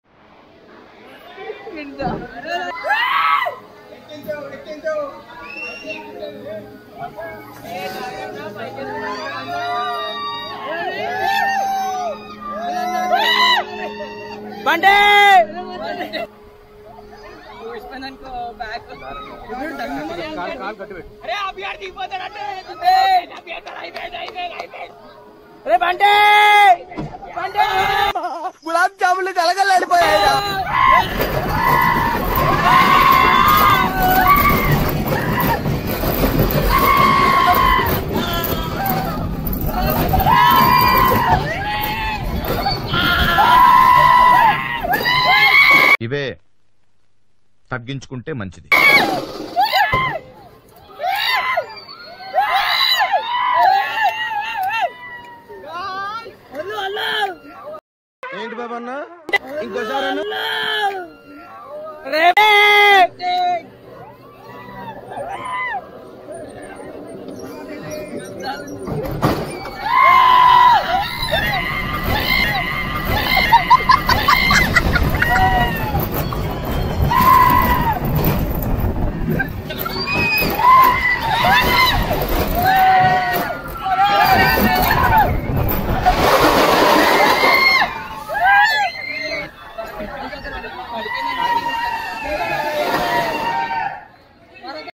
wonder la Amusement park 🎢 , Hyderabad
After 5 years with my school mates we went to wonder la .we don't the cam was fixed in front of us , after finishing that they shown to us and gave video with pendrive for some amount , such a amazing experience, no body had taken us when we are in school days. #2024